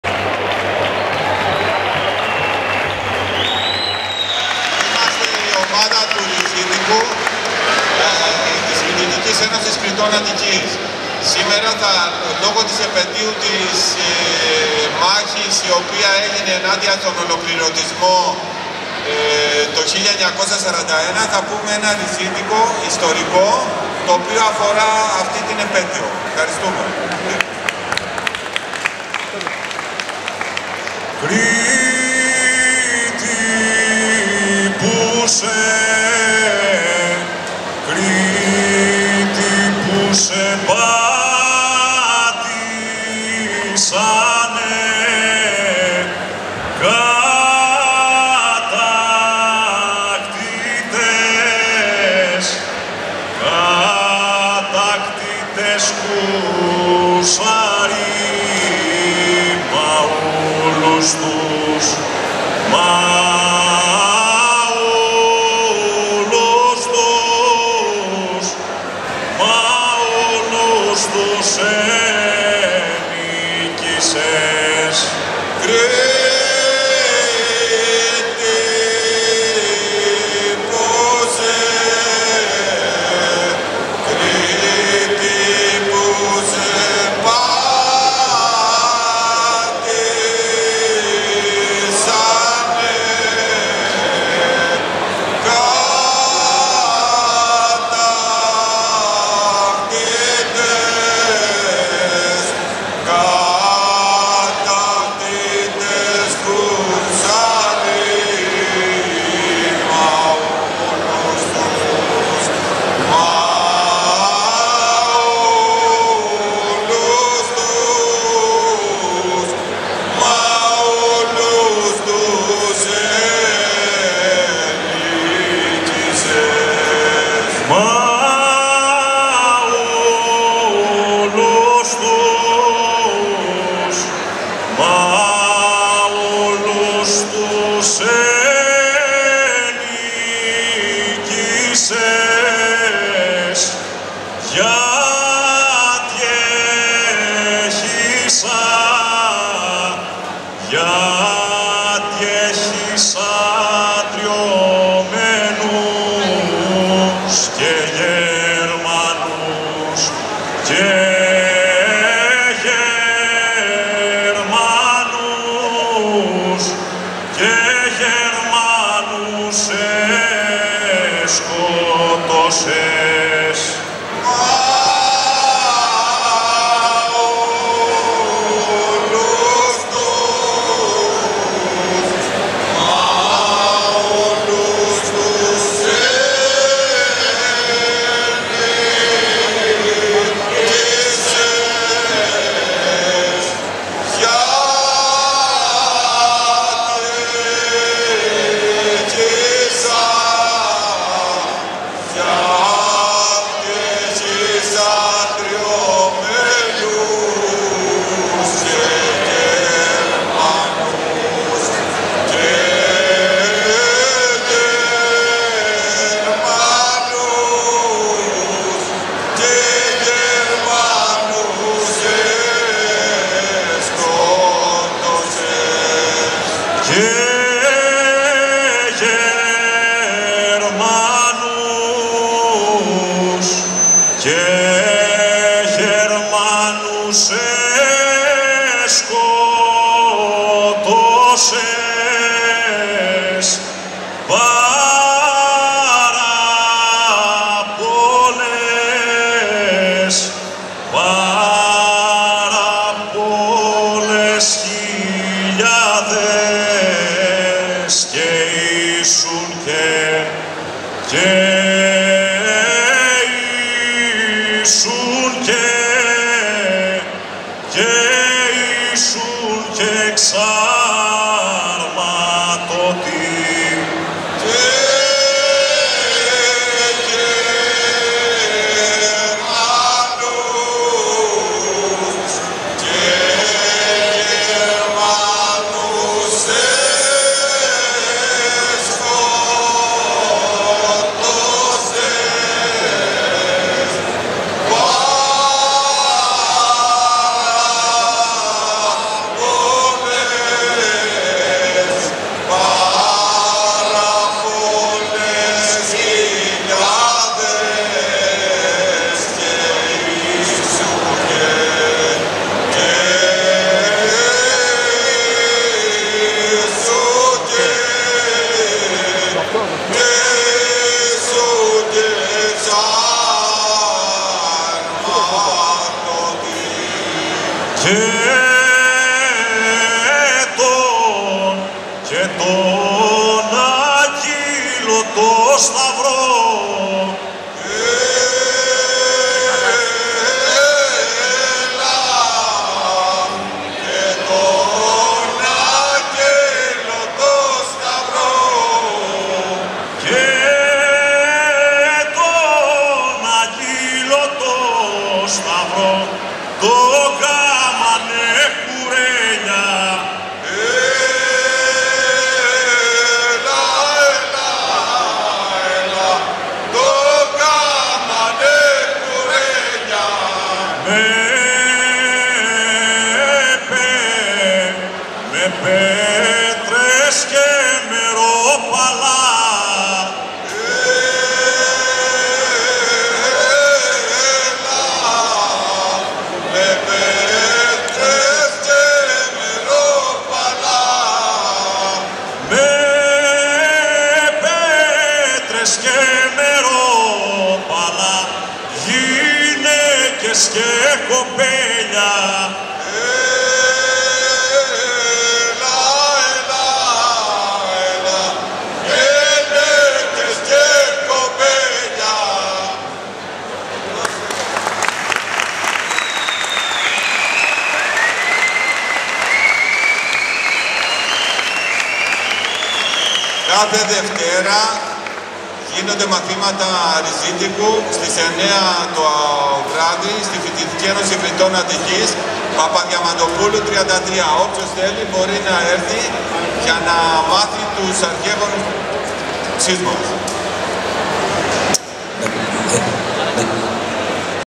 Φ.Ε.Κ. Αττικής - ''Ριζίτικο''
Υπεύθυνος Ριζίτικου: Μουζουράκης Ευτύχης
Εκδήλωση: Μεγάλο Εαρινό Γλέντι της Φοιτητικής Ένωσης Κρητών Αττικής στις 27 Μαΐου 2017 στο κλειστό γήπεδο / Πάρκο Γουδί του Δήμου Ζωγράφου.